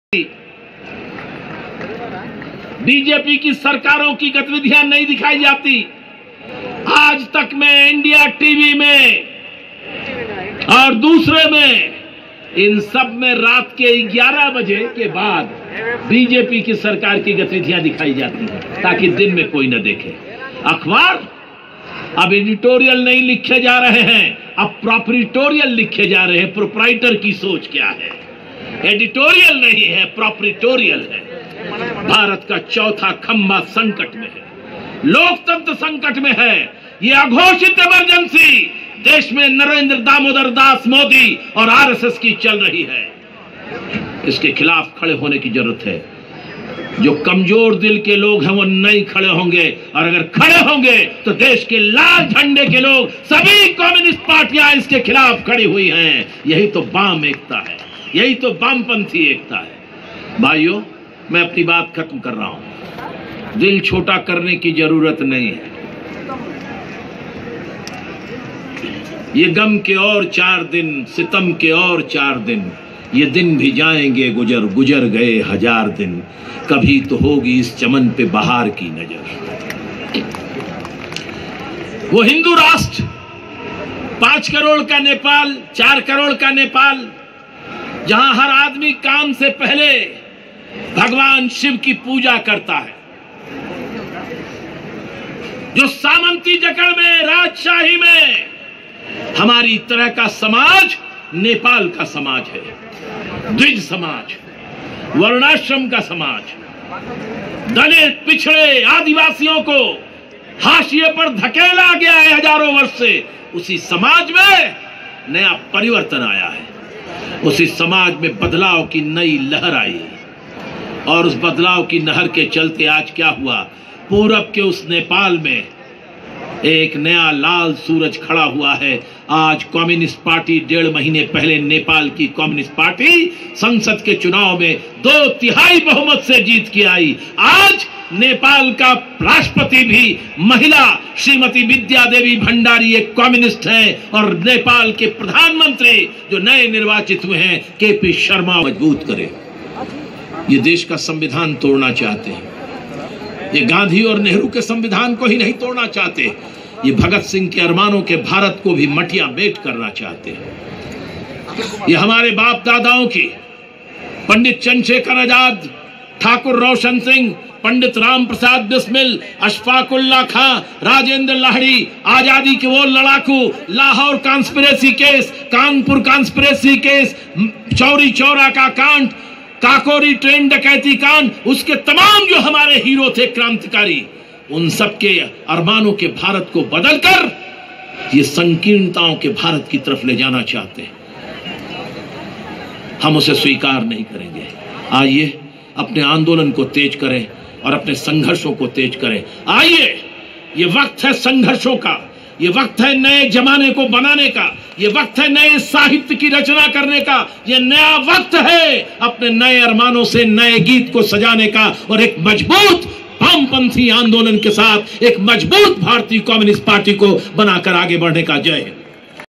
CPI TELANGANA STATE 2ND CONFERENCE,ALL INDIA GENERAL SECRETARY, S SUDAKAR REDDY
CPI TELANGANA STATE 2ND CONFERENCE,ALL INDIA GENERAL SECRETARY, S SUDAKAR REDDY,STATE SECRETARY CHADA VENKAT REDDY,DR K NARYANA,ATUL KUMAR,EX MP SYED AZIZ PASHA CITY SECRETARY ET NARSIMHA

HSTVNEWSCHANNEL
REPORTED by mohd hashmath
Cameramen santosh kumar